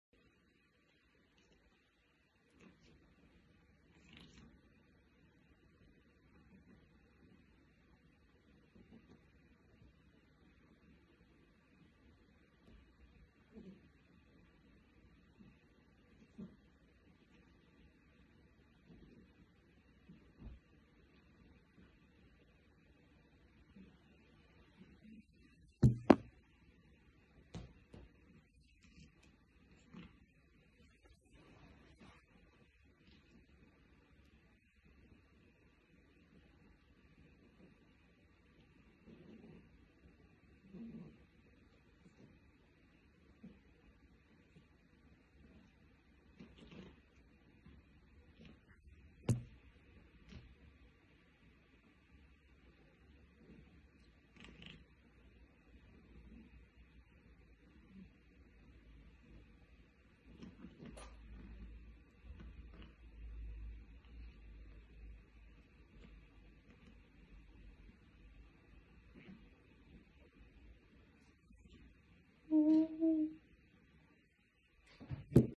Персидский серый кот
Персидский серый кот играет с ногами хозяина сидя на кровати